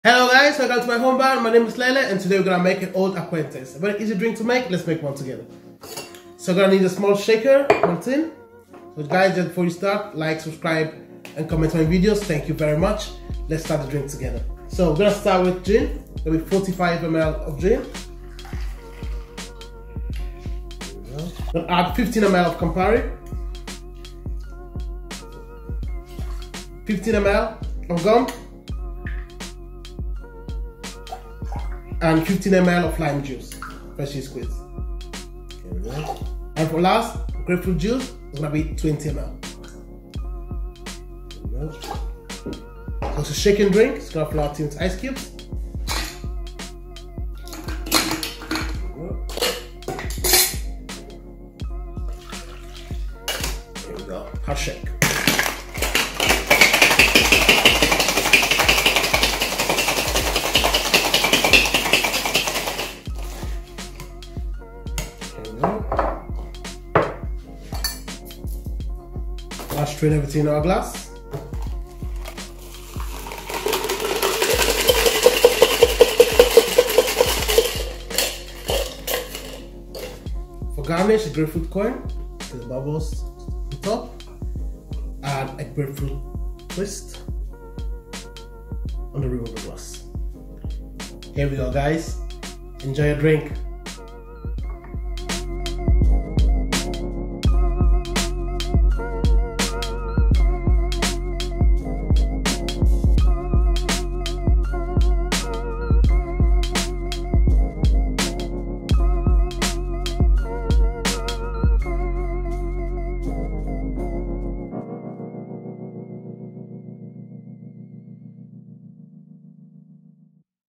How to make a OLD ACQUAINTANCE with LELE | COCKTAIL with GIN
INGREDIENTS:
45ml Gin
15ml Campari
15ml Gomme
15ml Lime Juice
20ml Grapefruit Juice

Garnish:
1 Grapefruit coin discard
1 Grapefruit peel

🎙CLUBHOUSE: @drinkwithlele